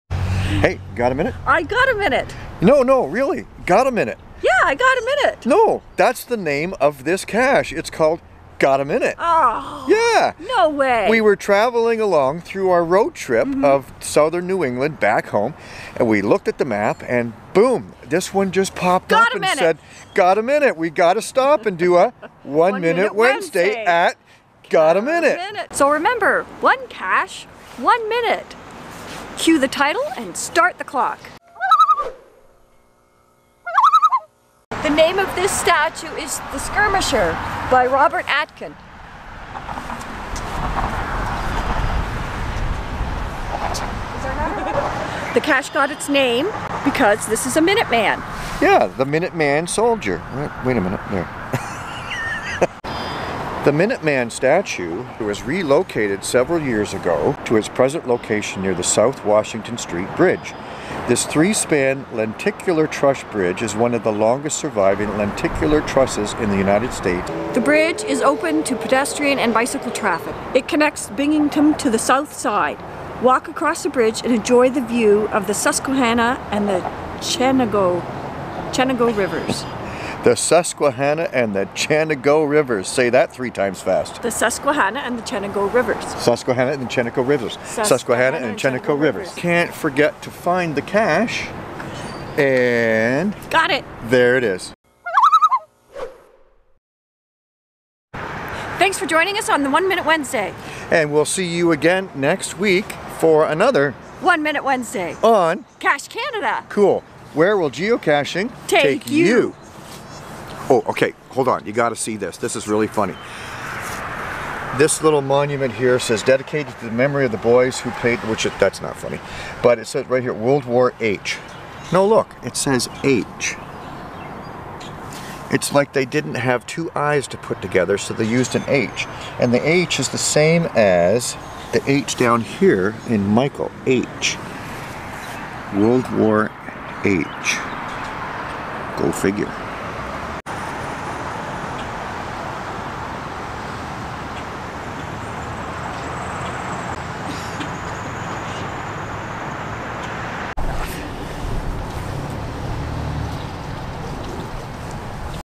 One Minute Wednesday - Got A Minute? No Really!! Got A Minute!?
Travelling home from our Southern New England Cache Tour we spotted a cache that we just HAD TO stop at. Watch and you will see why.Geocaching fun times.